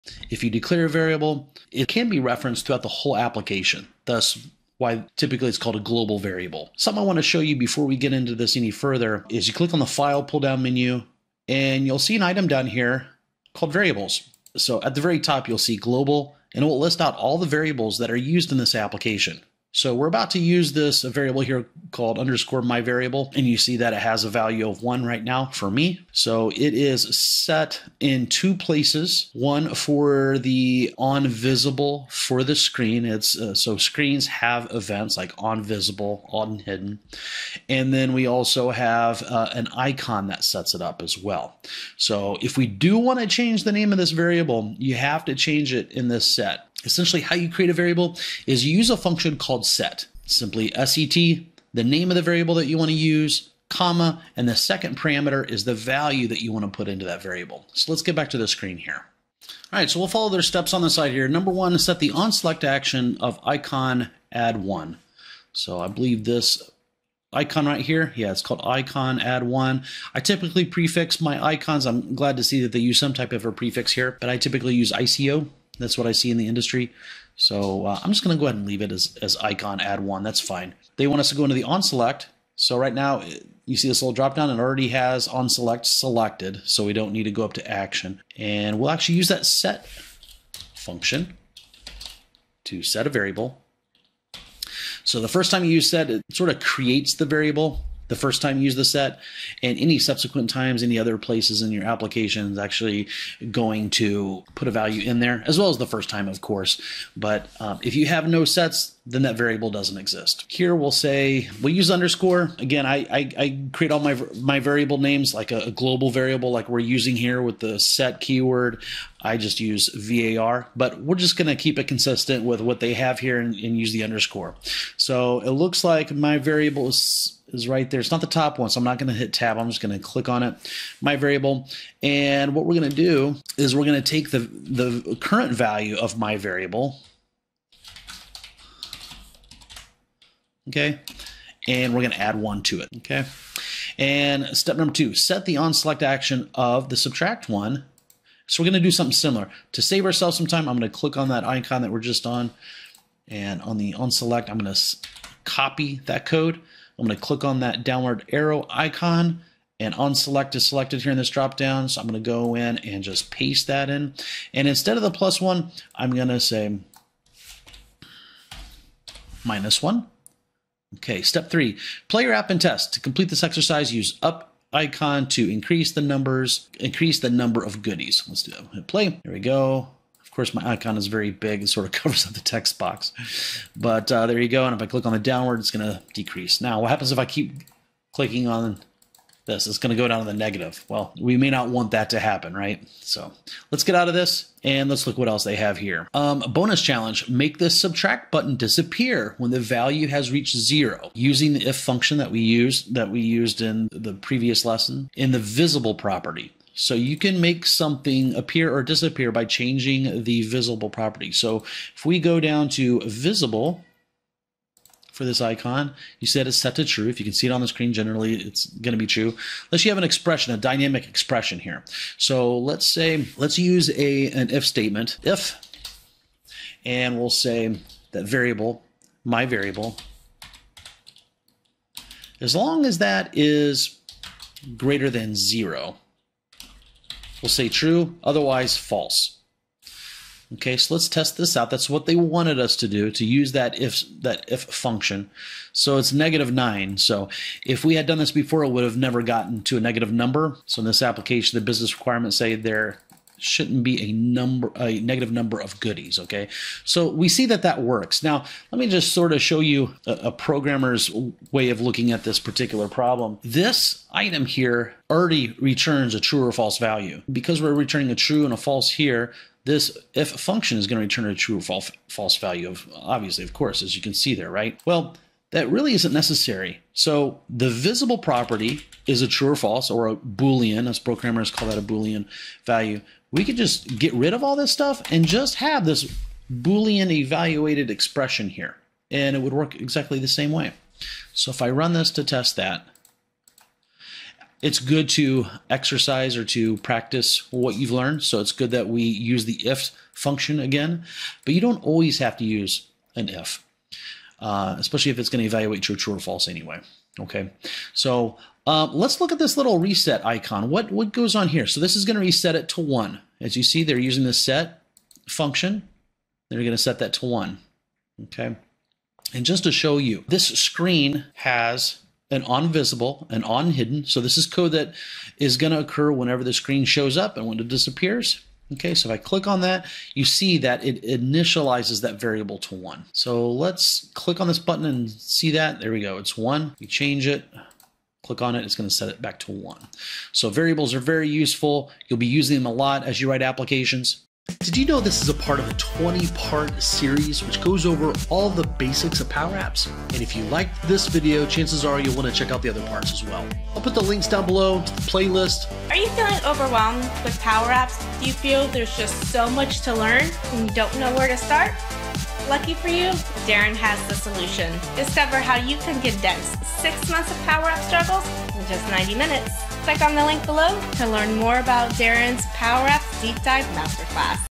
Power Apps Variables
⬅️ 🆓 Power Apps Variable: We will cover the use of global variables in Microsoft Power Apps, which includes the Set function.

Darren Neese, Microsoft MVP, PowerApps Tutorial